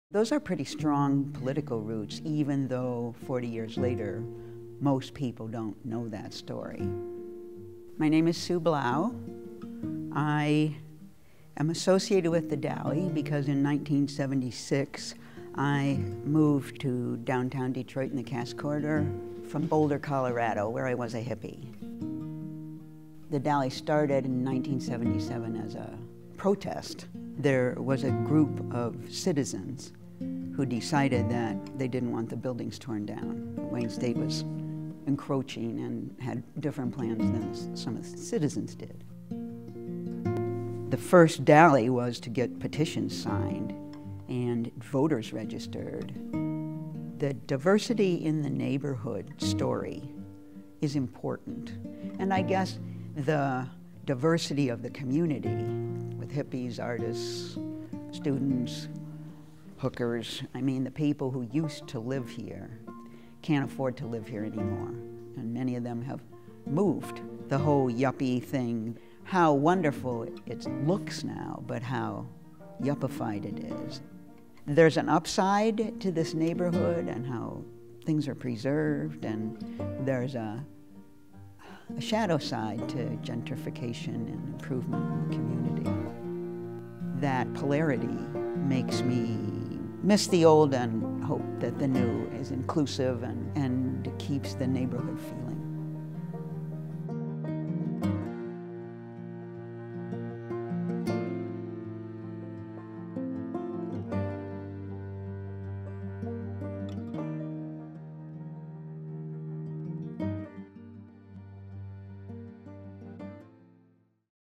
Dally in the Alley Mini Documentary
For a class project, I had to make a 2 minute video with footage shot by 6 people in 2 hours, and an interview with Sue Blouch. We covered the block party called "Dally in the Alley," and I chose to go with the themes of its political history, and the gentrification of the area it takes place.